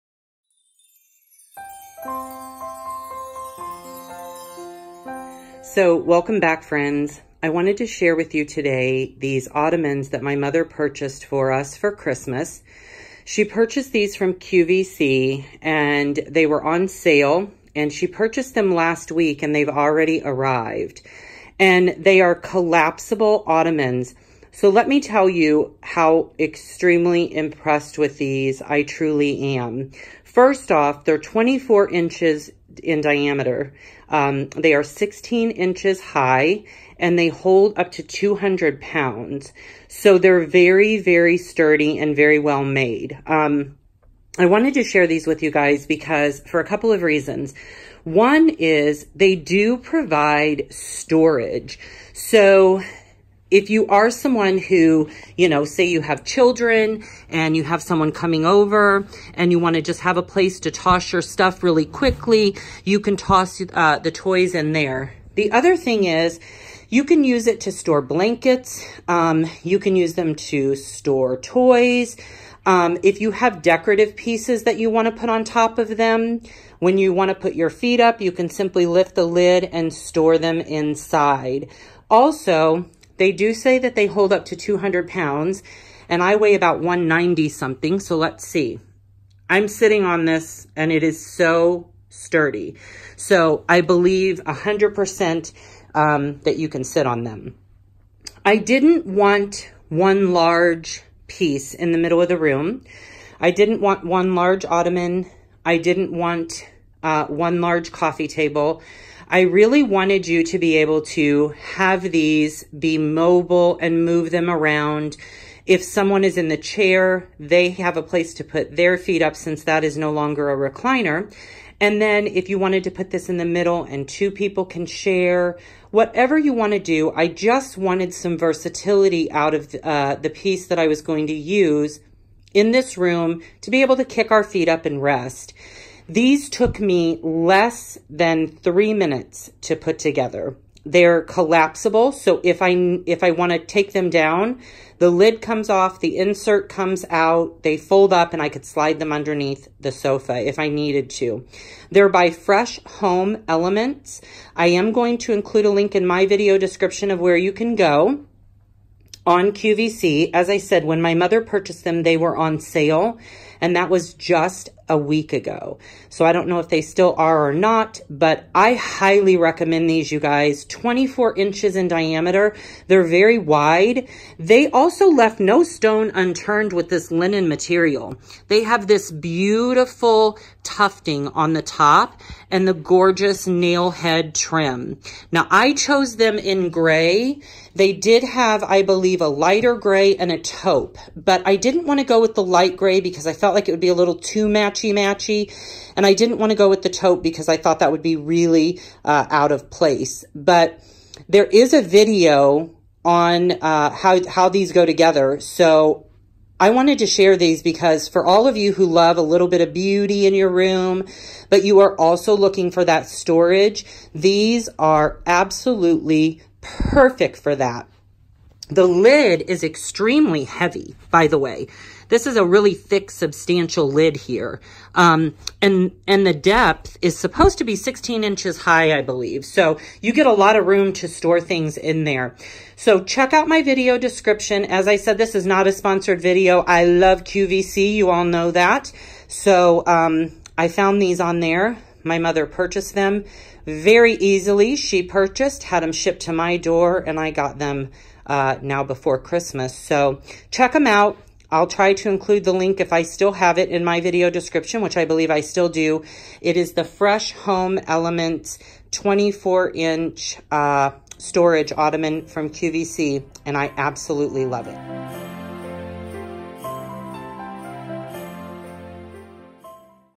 New Collapsible Storage Ottomans From QVC || Not a Sponsored Video!! || Beautiful & Functional
My mother purchased these from QVC as a Christmas Gift for us and I can’t say enough wonderful things about them!! (This is not a sponsored video)